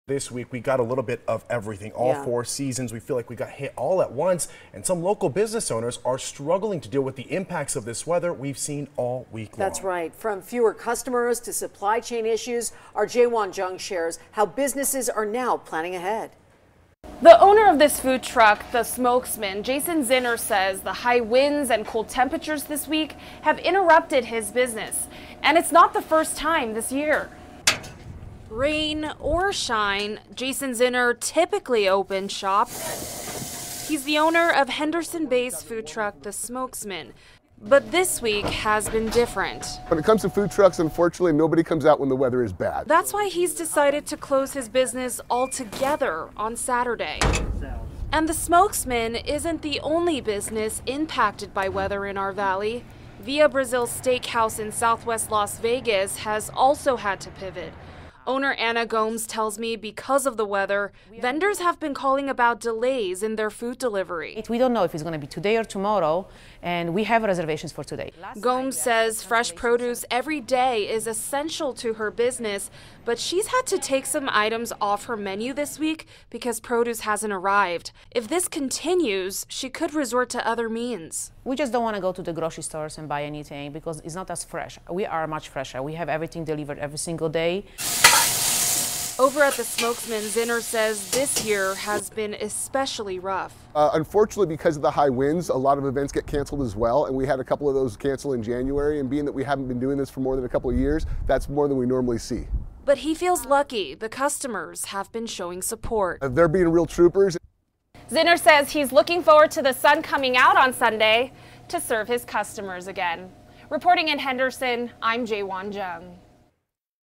Businesses forced to pivot as wild weather batters Las Vegas
Jaewon Jung shows us how the weather battering Las Vegas is impacting local businesses, forcing them to pivot and even temporarily close.